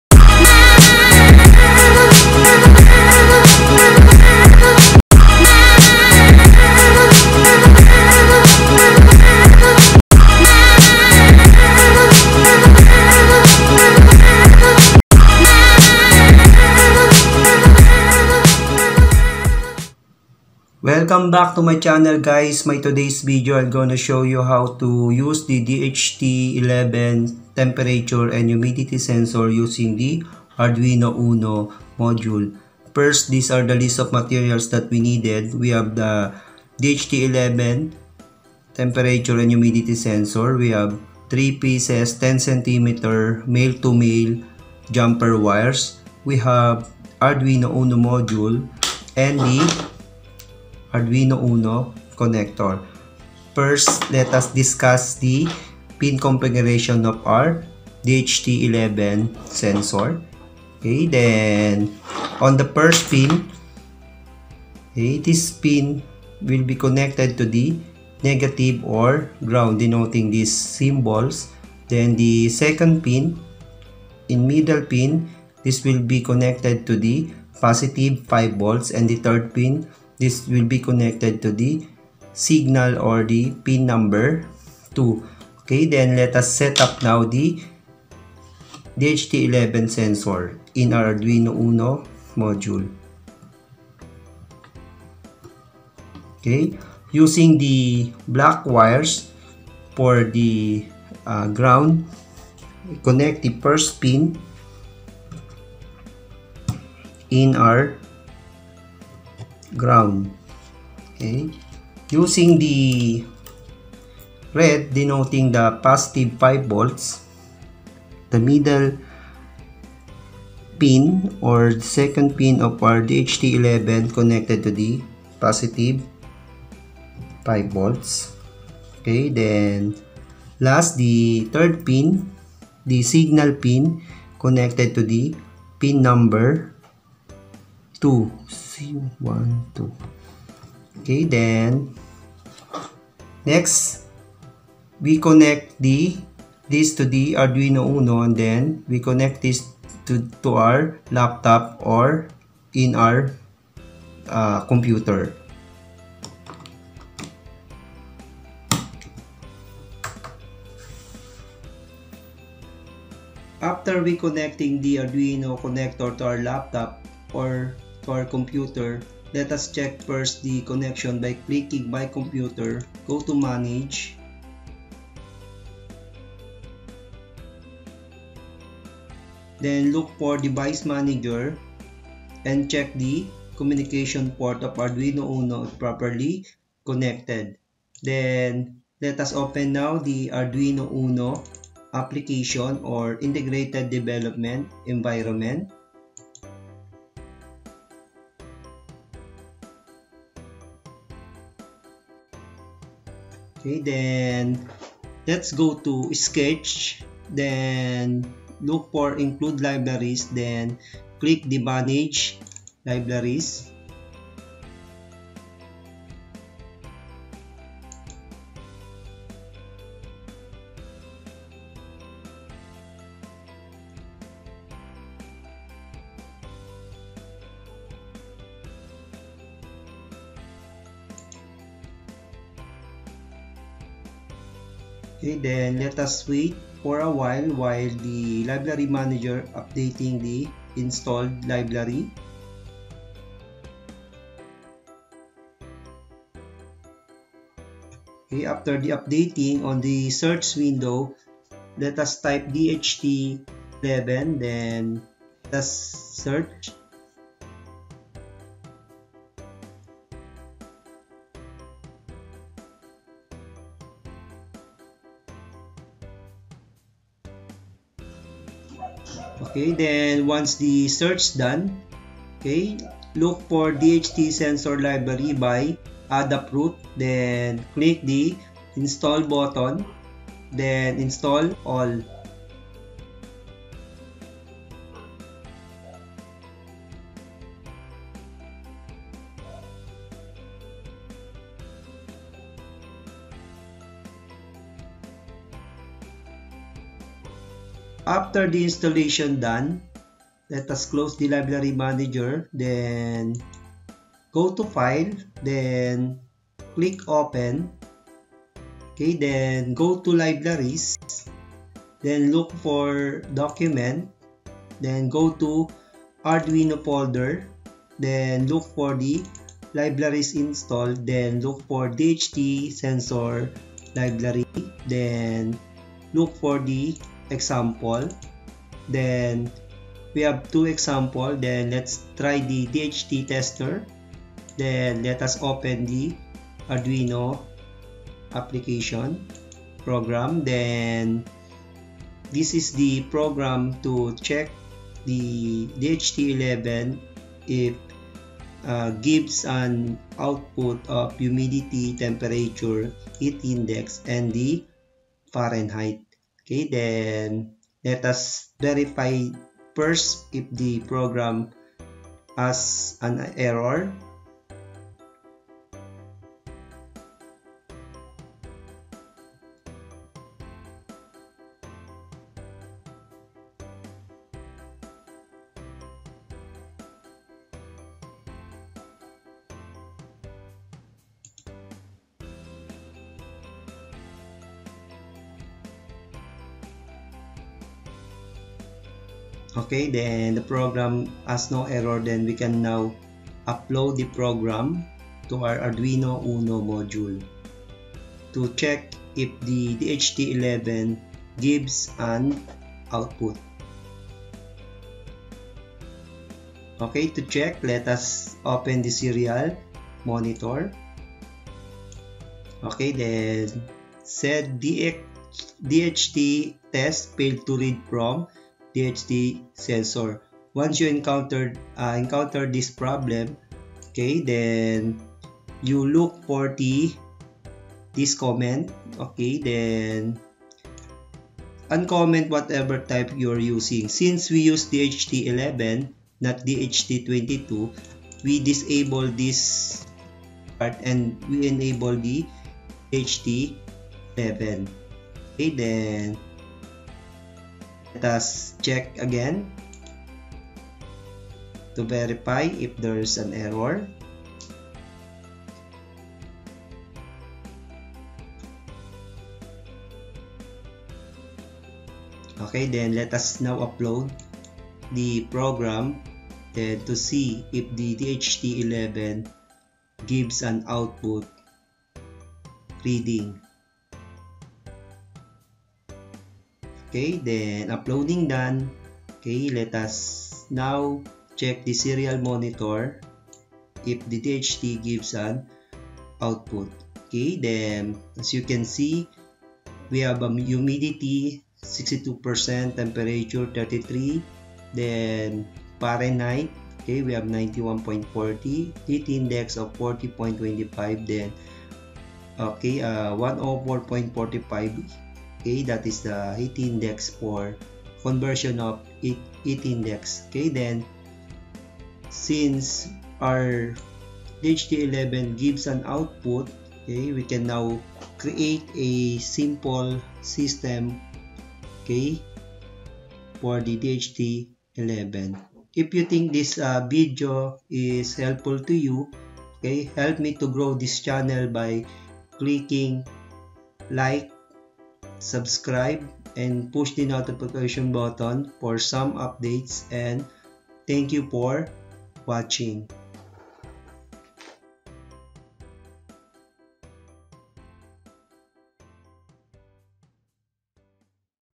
SET UP AND TEST DHT11 TEMPERATURE AND HUMIDITY SENSOR USING ARDUINO UNO MODULE
This video show how to set-up and test DHT11 using Ada fruit libraries in Arduino Uno module.